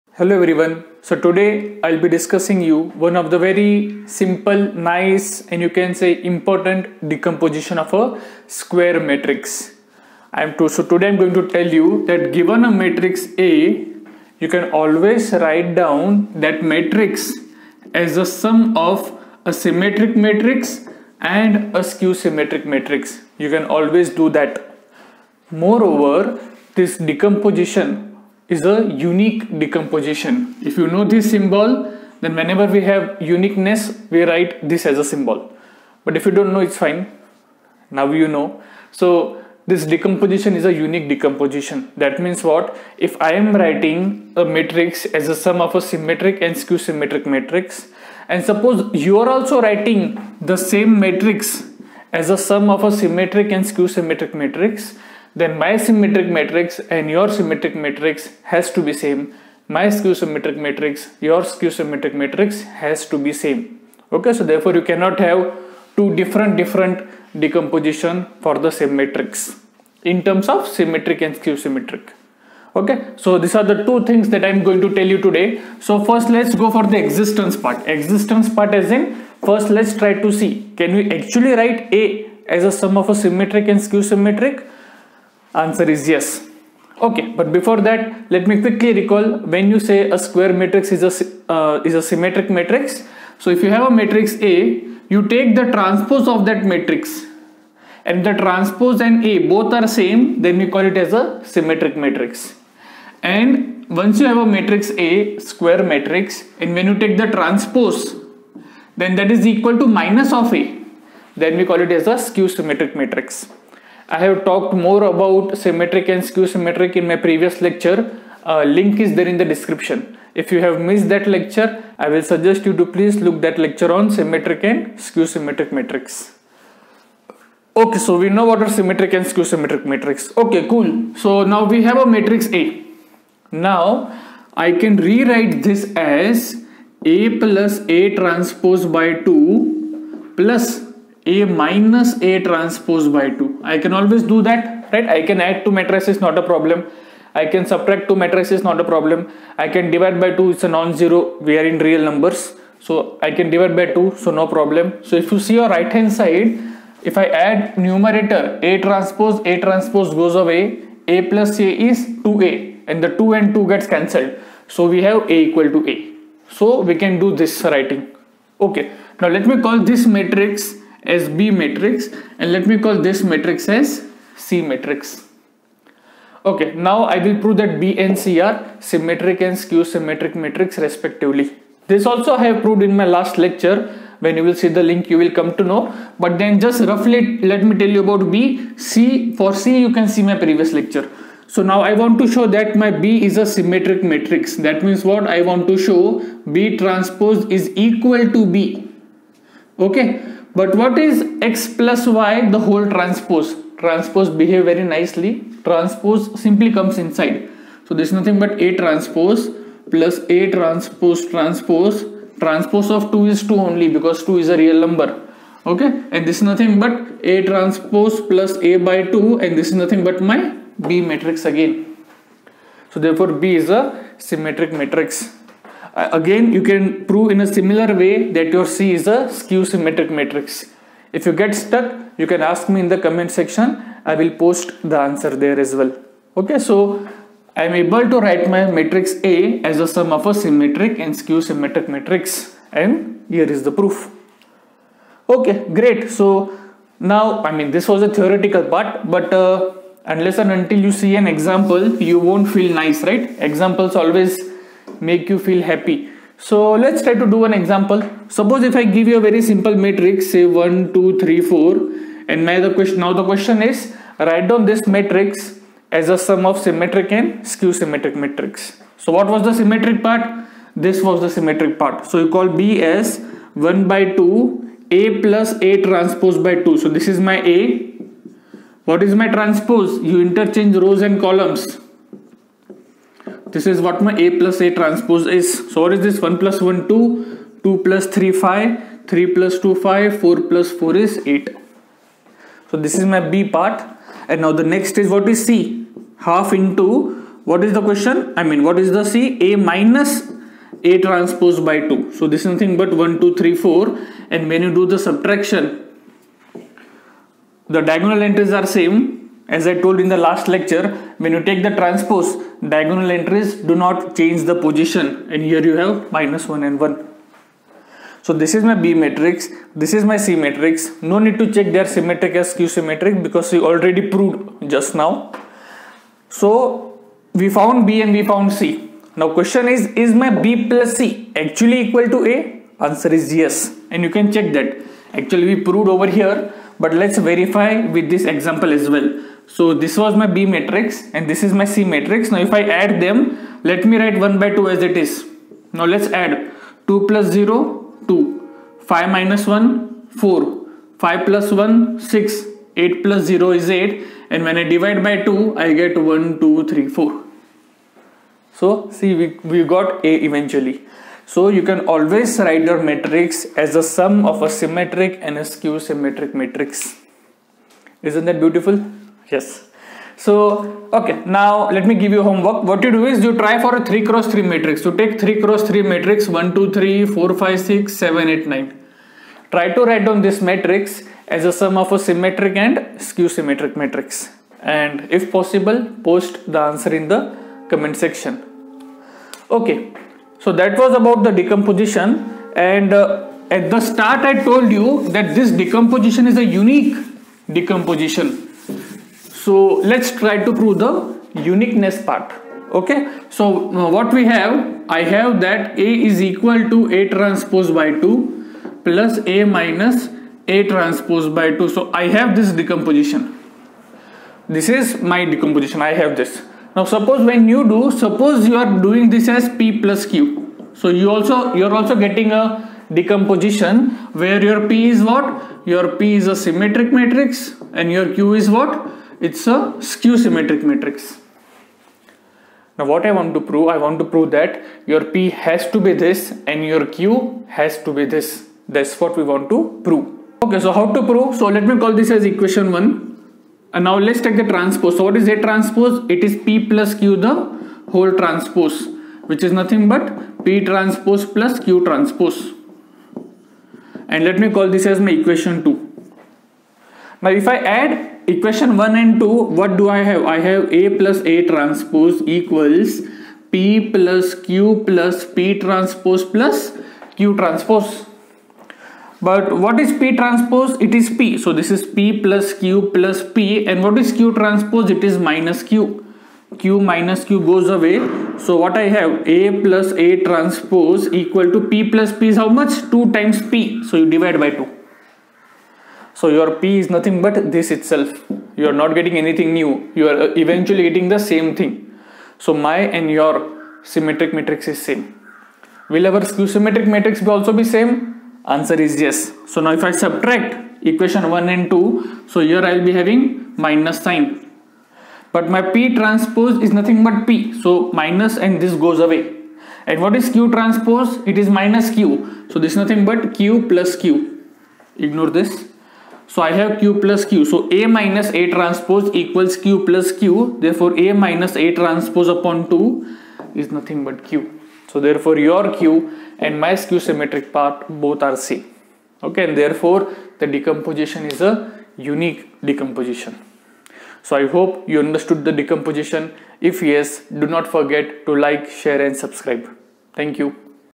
Any square matrix can be written as sum of Symmetric and Skew Symmetric matrix and that too UNIQUELY
In this video, we will see that any square matrix can be written as the sum of a symmetric and skew-symmetric matrix. Moreover, we will see that this decomposition is a unique decomposition.
We will prove both the facts. Moreover, we will also see examples of these facts.
We will start with the notion of Symmetric and skew-symmetric matrices and then we will proceed towards the proof.

Link for the previous lecture on tricks to find symmetric and skew-symmetric matrices.